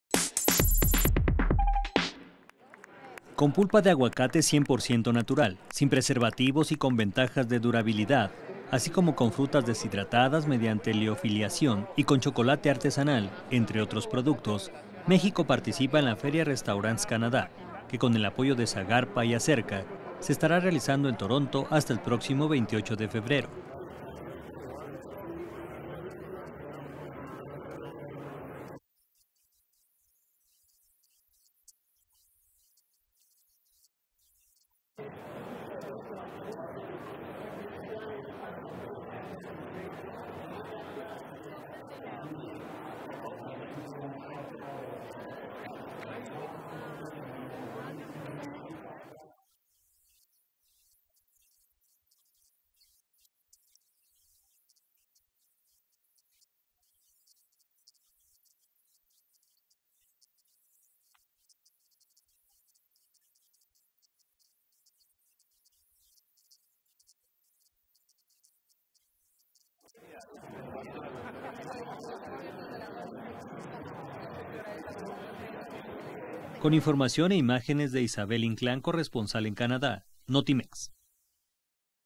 Sabor 100 por ciento mexicano en feria Restaurants Canada
México participa en la feria Restaurants Canadá con el apoyo de SAGARPA y ASERCA